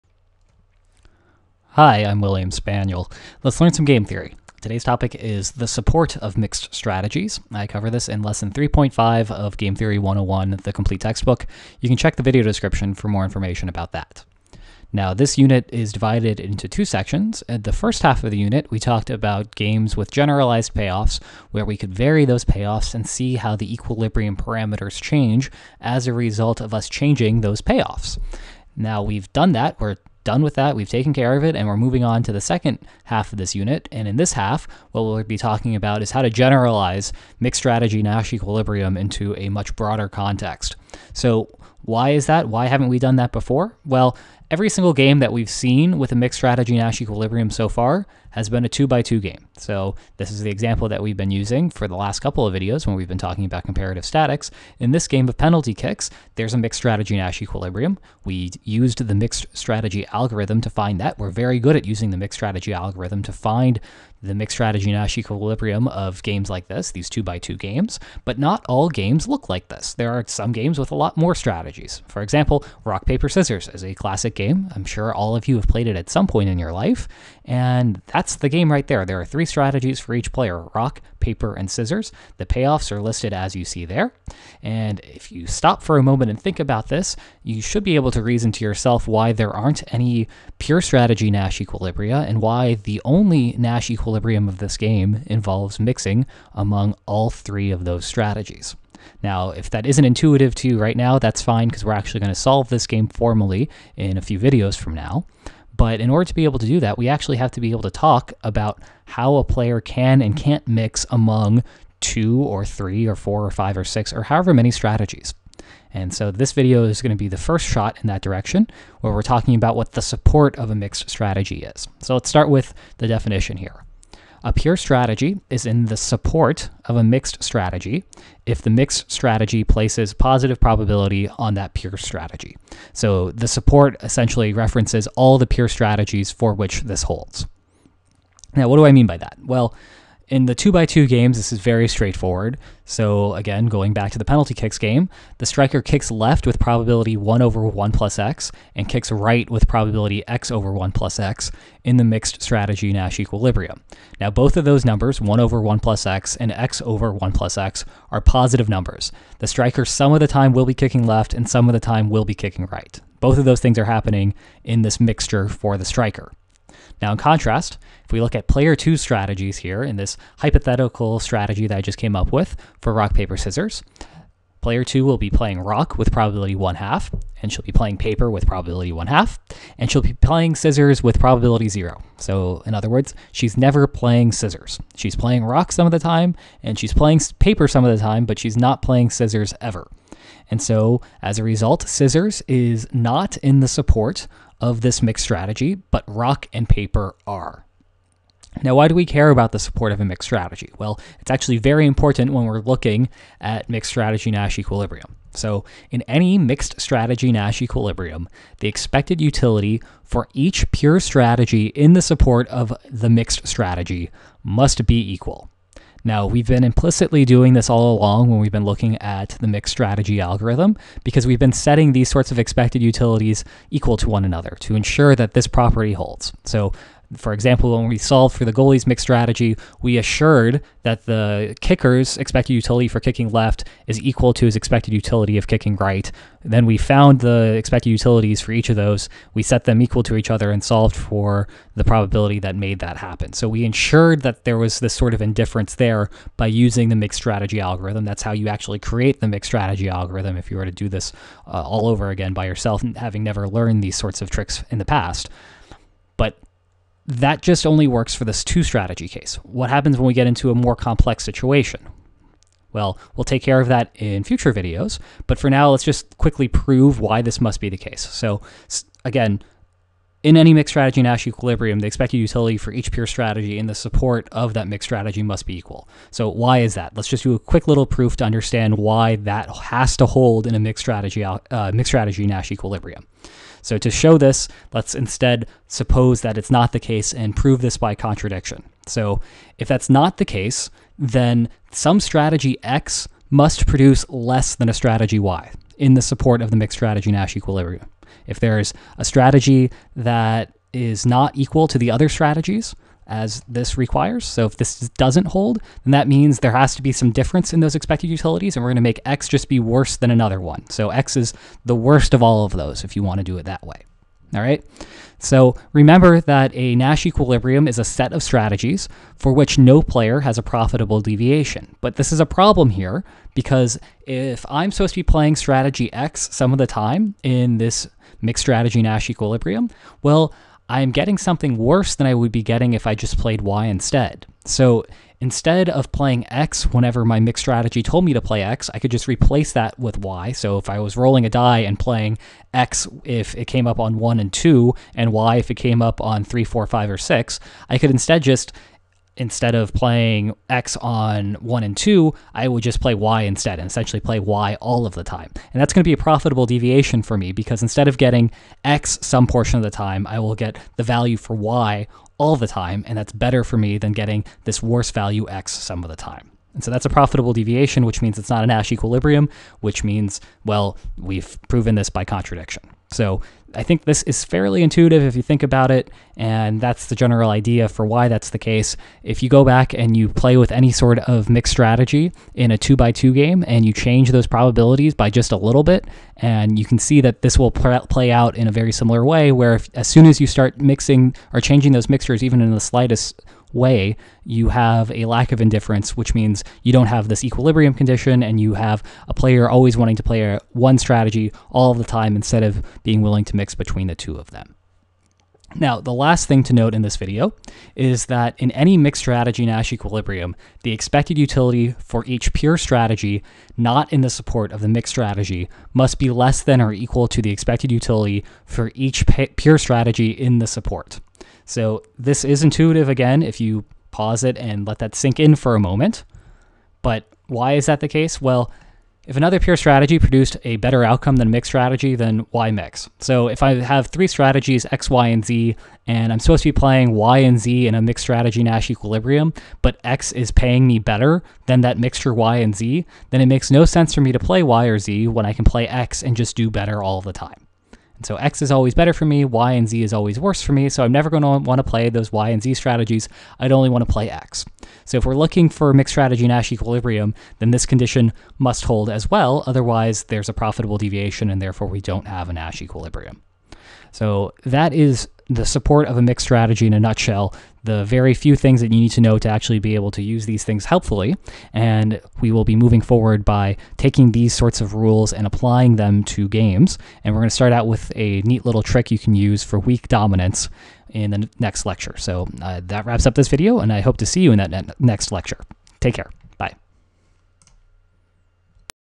Game Theory 101 (#32): The Support of Mixed Strategies
Our focus now shifts to understanding how mixed strategy Nash equilibrium functions beyond the scope of 2x2 games. This lecture covers the support of mixed strategies--that is, the pure strategies a player chooses with positive probability in his mixed strategies. Such strategies in the support must fulfill certain properties or they cannot be a part of a mixed strategy Nash equilibrium.